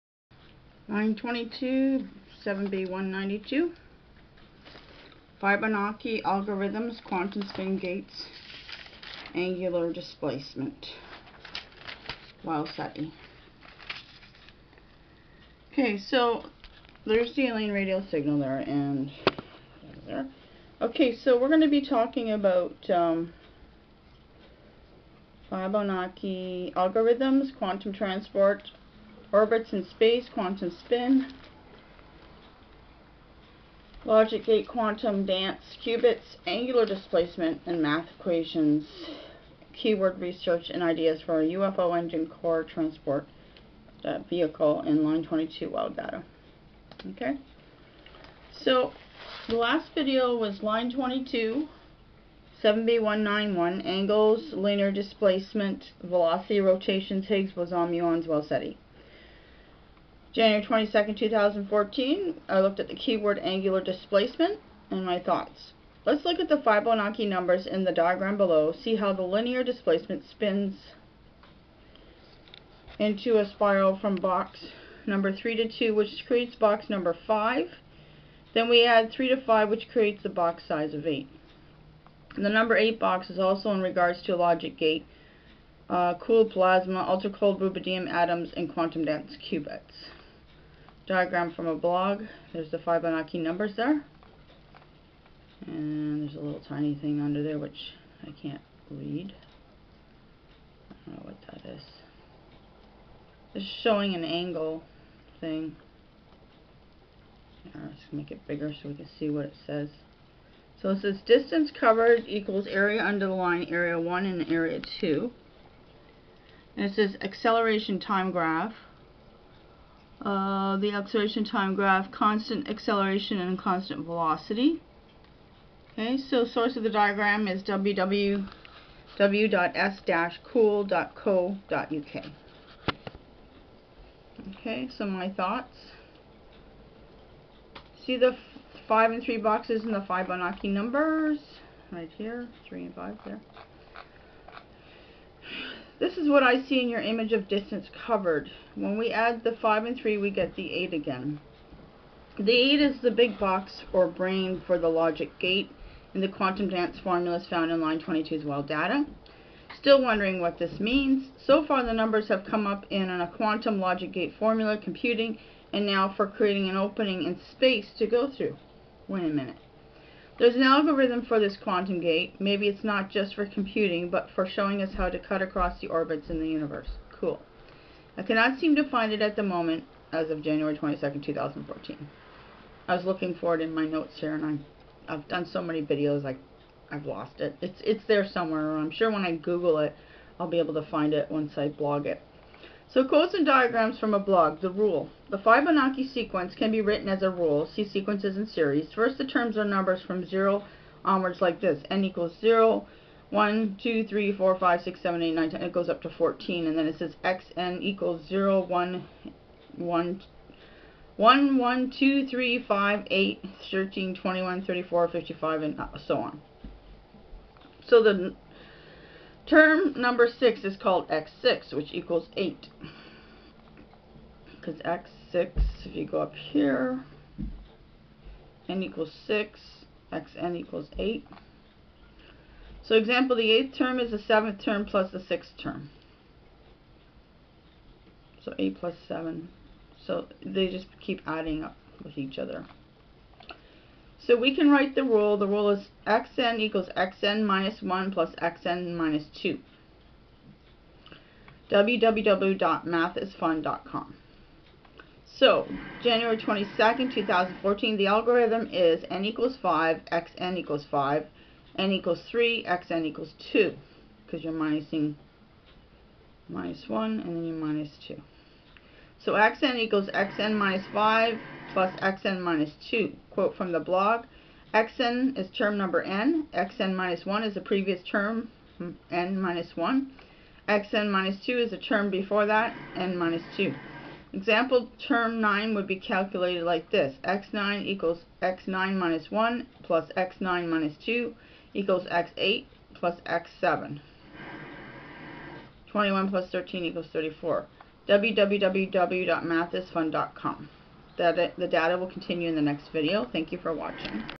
Line 22 7b192 Fibonacci Algorithms Quantum Spin Gates Angular Displacement WOW SETI
7b97z 192 of 100 videos there are more videos after this one #170 wont upload.
 i'll post all then update the #.
11/111/1/1/14=0.0071
Google 0.0071

Fibonacci algorithms quantum transport orbits in space, quantum spin logic gate quantum dance qubits, angular displacement math equations and keyword research and Ideas for UFO engine Core Transport in line 22 wow data.

To be filmed 27 jan 2014
Filmed 30 jan 2014
Line 22 7b191 Angles Linear Displacement Velocity Rotations HIGGS Boson Muons WOW SETI

22 Jan 2014

 angular displacement.

My thoughts:

Let's look at the Fibonacci Numbers in the diagram below see how the linear displacement spins into a spiral from box number 3 to 2 which creates box number 5  than we add the 3 to 5 which creates the box size of 8.

The number 8 box is also in regards to logic gate+ cooled plasma + ultra cold rubidium atoms + quantum dance + qubits

My thoughts:

 see the 5 and 3 boxes in the Fibonacci numbers?

This is what I see in your image of distance covered.

When we add the 5 and 3 we get the 8 again!

The 8 is the big box or brain for the logic gate in the quantum dance formula's found in line 22's wow data?

Still wondering what this means...

So far the numbers have come up in an quantum logic gate formula (computing) and now for creating an opening in space to go through!

There's an algorithm for this quantum gate, maybe its not just for computing but for showing us how to cut across the orbits in the universe - cool!

I cannot seem to find it at the moment (jan 22 2014)

Quotes diagrams from a blog:

The Rule

The Fibonacci Sequence can be written as a "Rule" (see Sequences and Series).

First, the terms are numbered from 0 onwards like this:
n =
0
1
2
3
4
5
6

So term number 6 is called x6(which equals 8.

So we can write the rule:

The Rule is xn = xn-1 + xn-2

22 Jan 2014

The algorithm is n = 5 Xn =5   n =3 Xn = 2

So Xn = Xn -5 + Xn -2

Quote from a blog:

where:
 • xn is term number "n"

Example: term 9 would be calculated like this:
x9 = x9-1 + x9-2 = x8 + x7 = 21 + 13 = 34

Data continues in next video